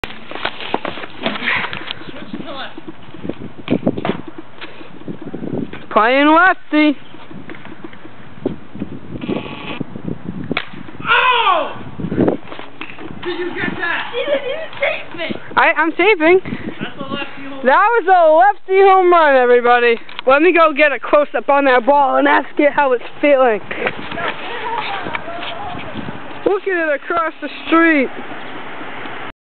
Switch Hit Home Run
This is a video of me and a friend playing wiffleball, and for fun I switched over to the left side to bat.